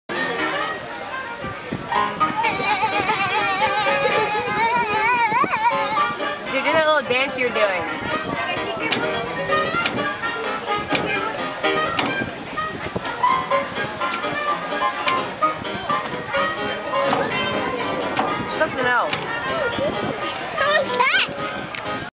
Steamboat Natchez
Jude getting carried away on the Steamer Natchez